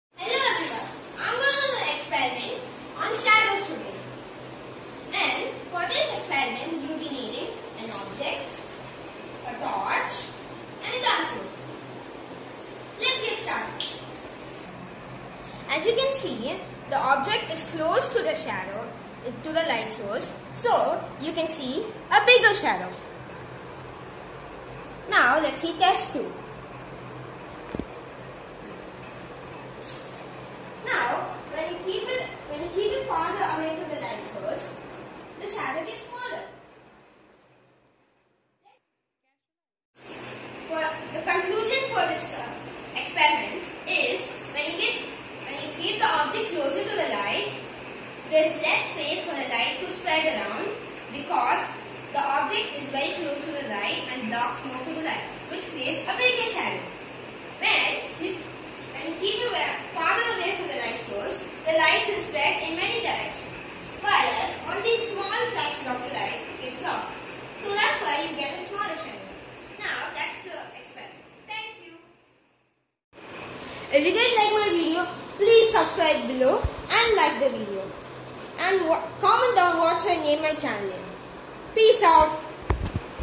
really cool and easy shadow experiment!
Today I did a shadow experiment on why shadows got bigger when the object got bigger.

Omar Faris
Andrew D ark